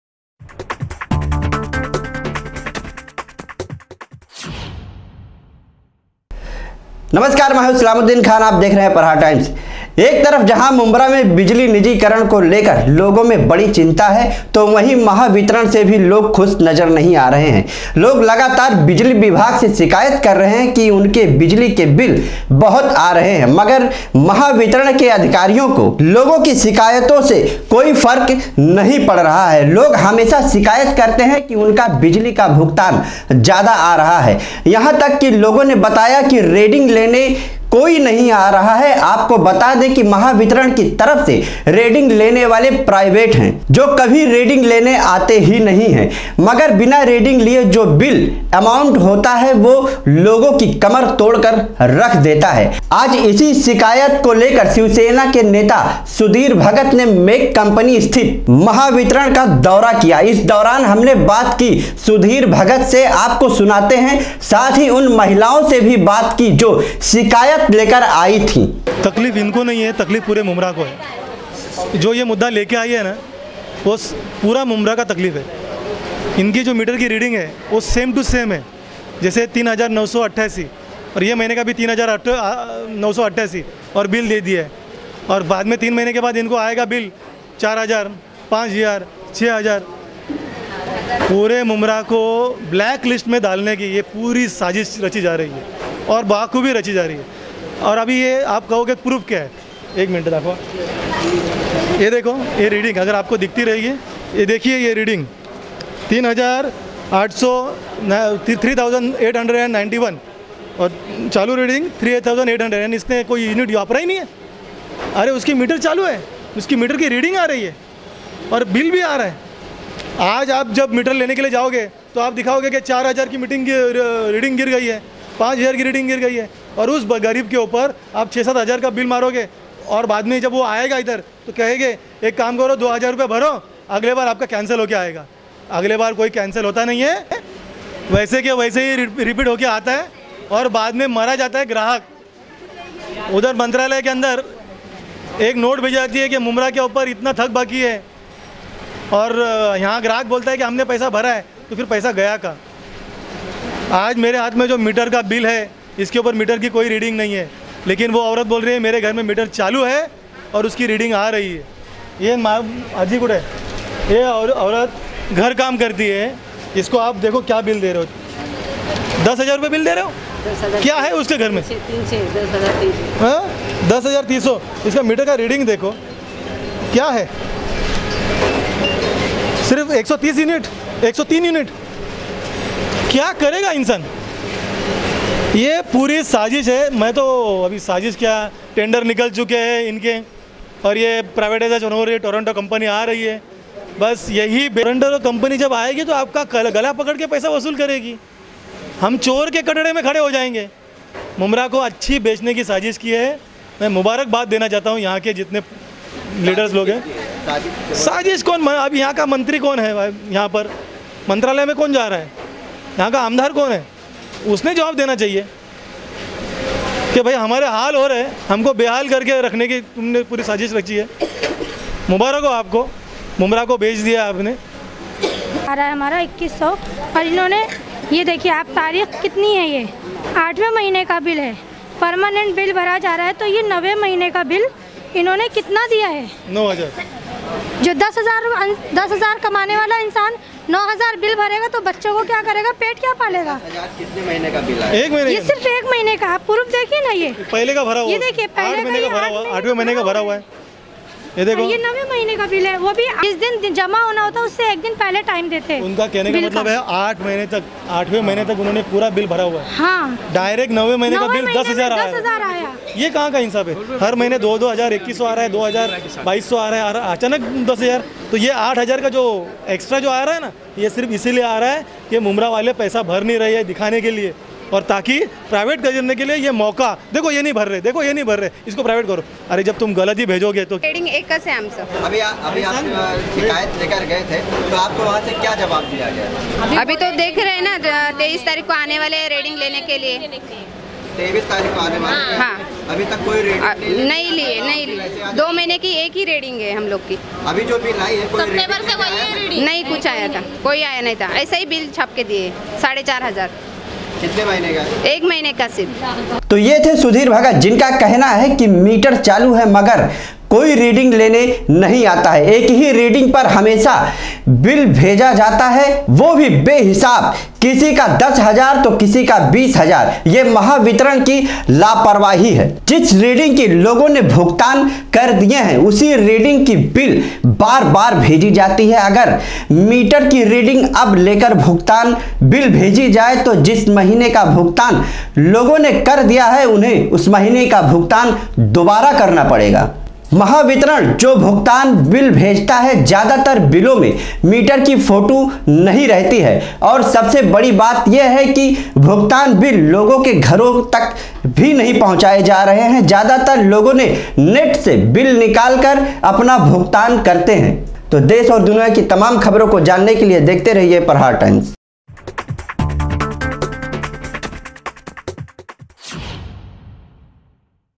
मुंब्रा के लोग बिजली बिल से परेशान ,लोगों की शिकायत इतना ज्यादा बिल आता है कैसे कोई भुगतान करे
मुंब्रा के लोग बिजली बिल की समस्या से परेशान ,
लोगो ने बताया 10 से 15 हजार महीने का बिल आता है कैसे कोई भरेगा

सुधीर भगत ने मेक कंपनी स्थित महावितरण का दौरा किया,जिसमे महावितरण की लाप्रवाही सामने आई

prahar times